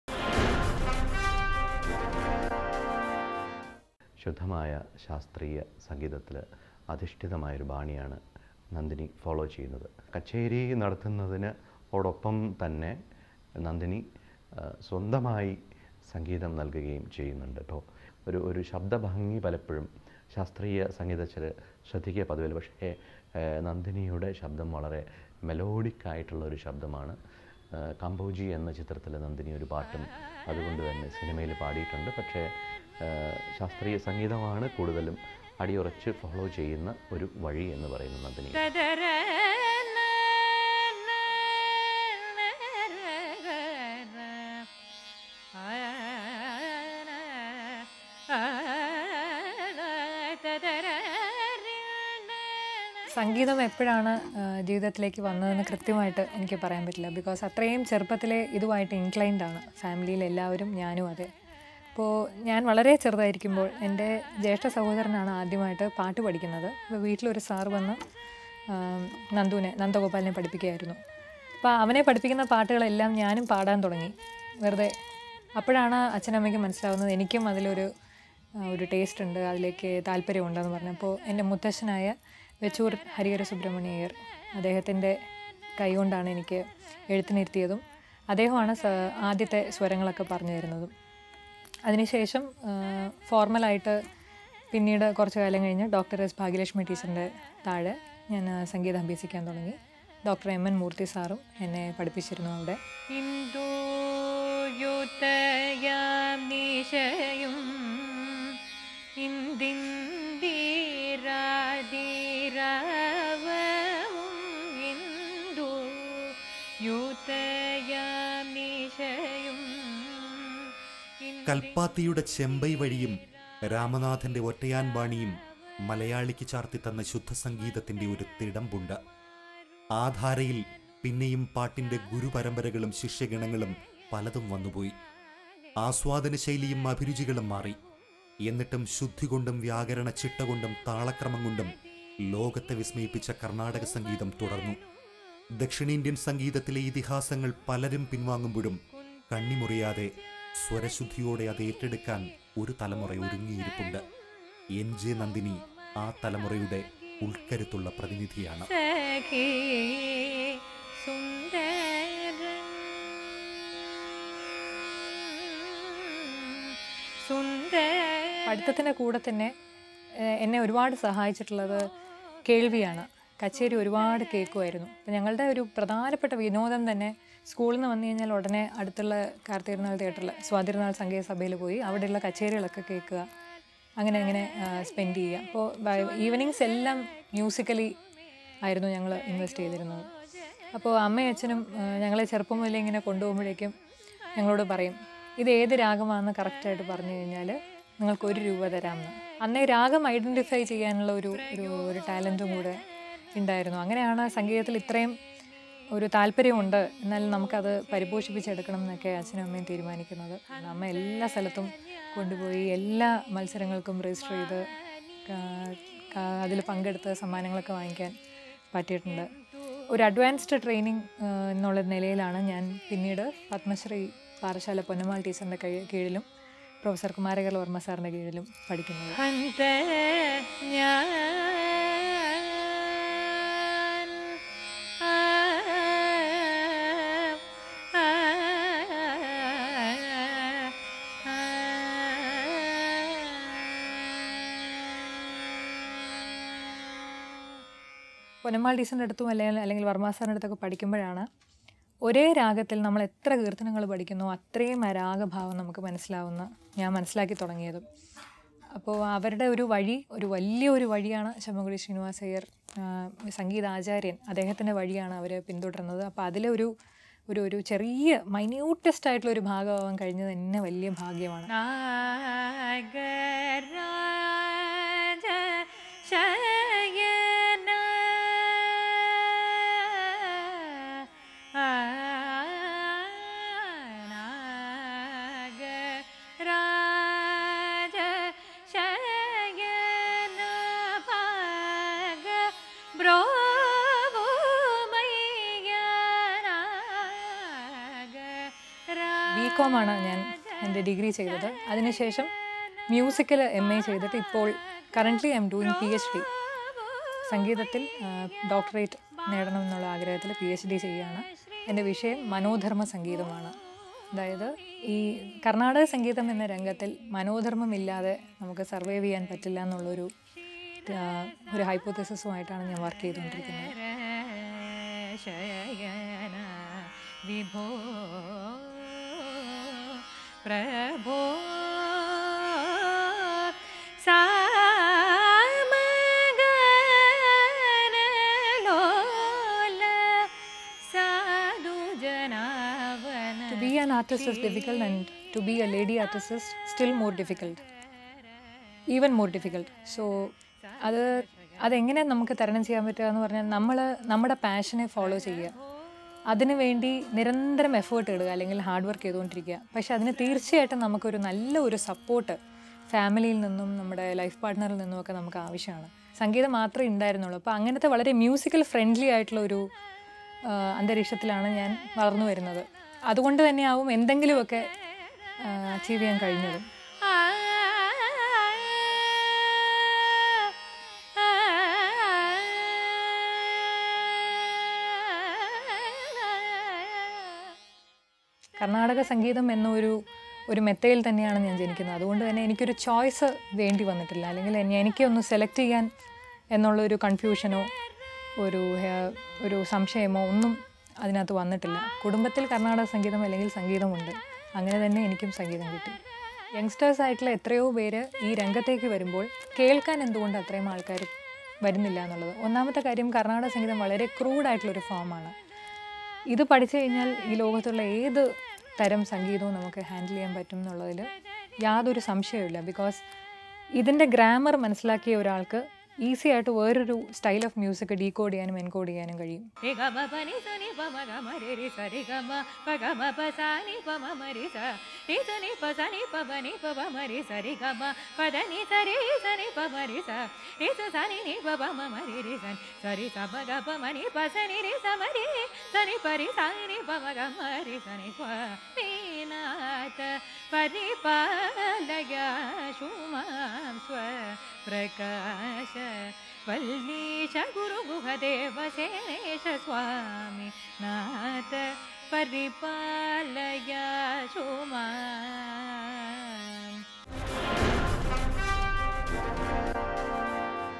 N. J. Nandini | SthreeShakthi Award 2018 contestant Music category
N. J. Nandini | Sthree Shakthi Award 2018 contestant Music category
N J Nandini is a young Carnatic music vocalist from Trivandrum. 2014 was a high note for Nandini, who has become the young face of classical music in the city.